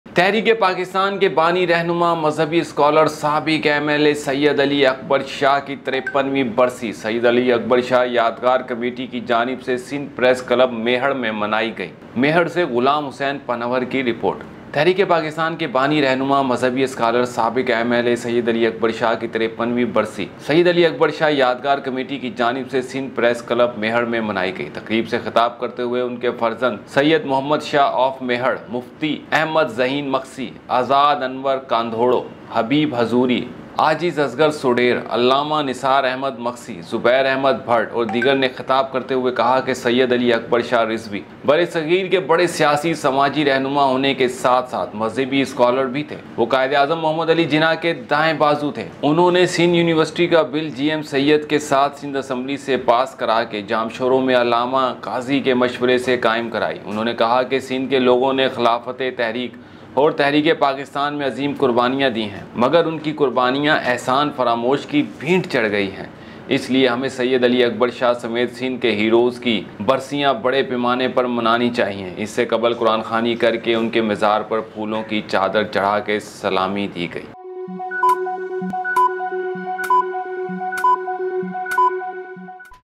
Sindh |تحریک پاکستان کے بانی رہنما , مذہبی اسکالر , سابق ایم ایل اے سید علی اکبر شاہ کی 53 ویں برسی
Sindh News |
تحریک پاکستان کے بانی رہنما , مذہبی اسکالر , سابق ایم ایل اے سید علی اکبر شاہ کی 53 ویں برسی سید علی اکبر شاہ یادگار کمیٹی کی جانب سے سندھ پریس کلب میہڑ میں منائی گئی